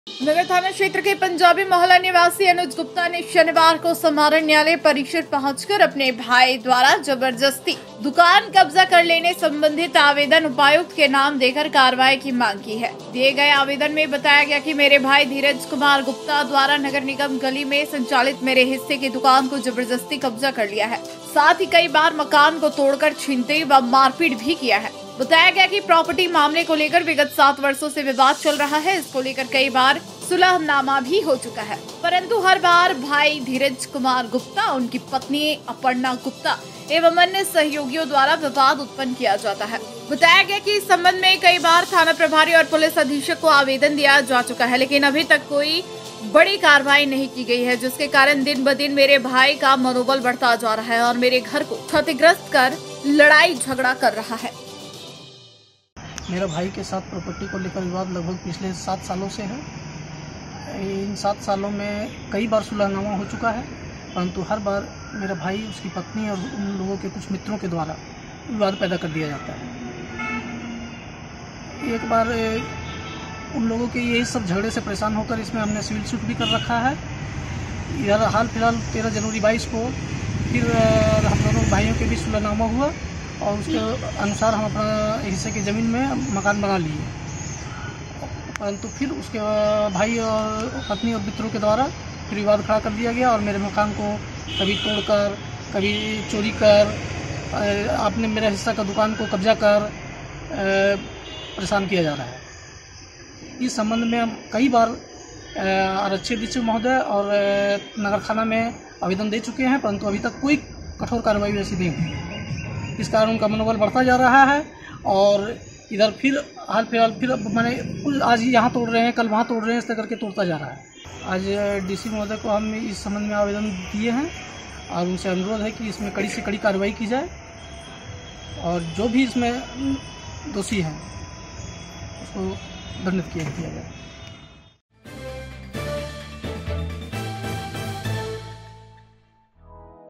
Giridih भाई द्वारा जबरदस्ती दुकान कब्जा कर लेने संबंधित आवेदन देकर कार्रवाई की मांग की
नगर थाना क्षेत्र के पंजाबी मोहल्ला निवासी अनुज गुप्ता ने समाहरणालय परिसर पहुंचकर अपने भाई द्वारा जबरदस्ती दुकान कब्जा कर लेने संबंधित आवेदन उपायुक्त के नाम देकर कार्रवाई की मांग की है।